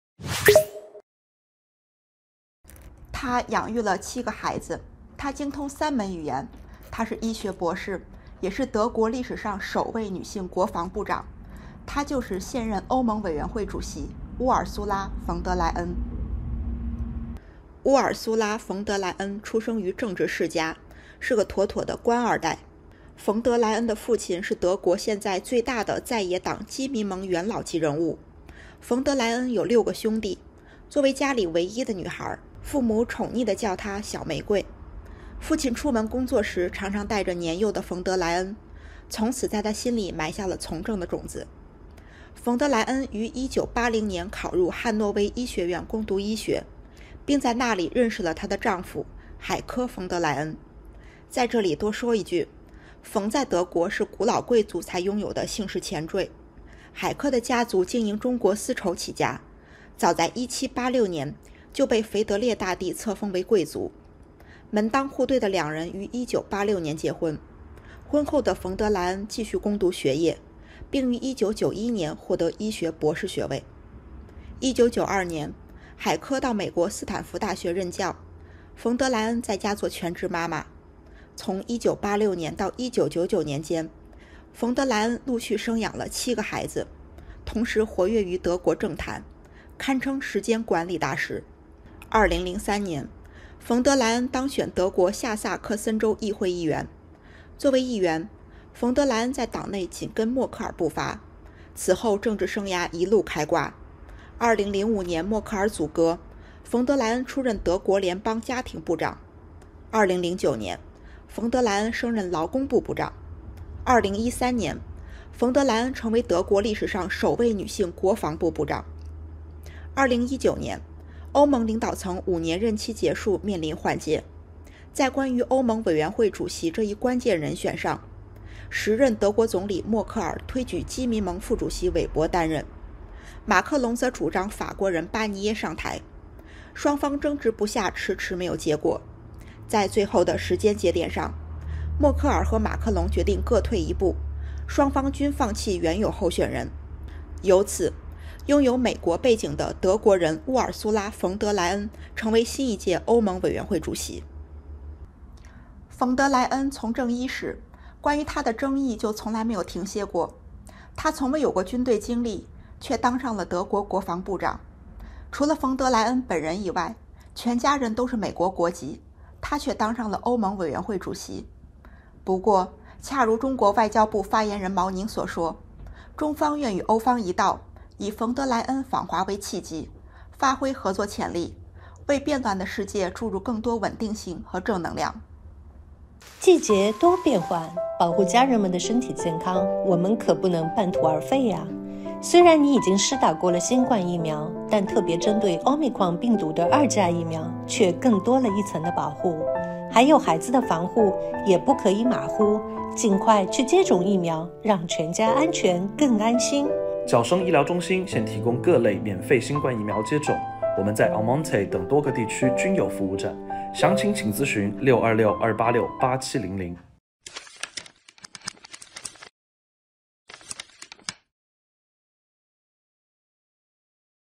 拥有7个孩子的女性政坛领袖 欧盟委员会主席——乌尔苏拉冯德莱恩
她养育了7个孩子；她精通3门语言；她是医学博士，也是德国历史上首位女性国防部长。她就是现任欧盟委员会主席——乌尔苏拉冯德莱恩。